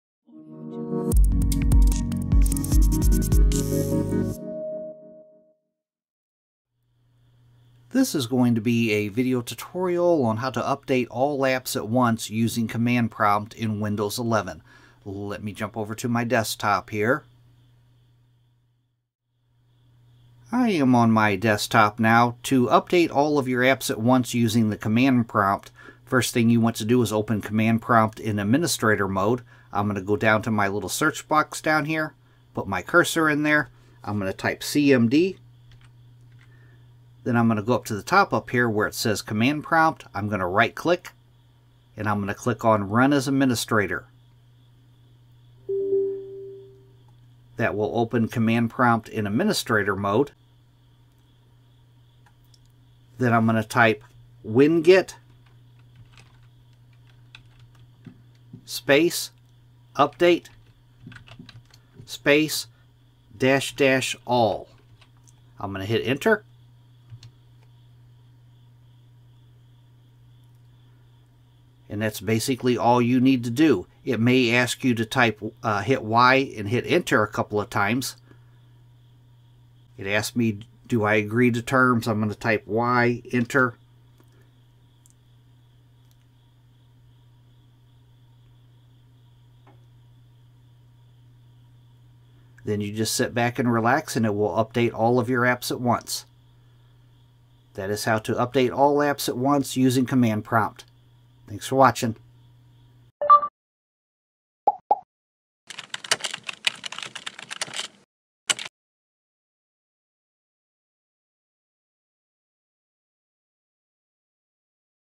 How To Update All Files At Once Using Command Prompt In Windows 11: Step-by-Step Guide!
In this tutorial, you’ll learn how to update all files at once using Command Prompt in Windows 11. This method is useful when you need to quickly refresh, replace, or copy updated versions of multiple files in bulk without having to handle them one by one.

Simple Steps
1. Open Command Prompt In Administrator Mode
2. Type the command winget update --all And Hit Enter
3. You may be asked to type y and hit enter a couple time.

Chapters
0:00 How To Update All Programs At Once Using Command Prompt
0:25 How To Run Command Prompt As Administrator In Windows 11
0:52 Type winget update --all And Hit Enter
1:10 You May Be Asked To Agree To Terms, Type Y And Hit Enter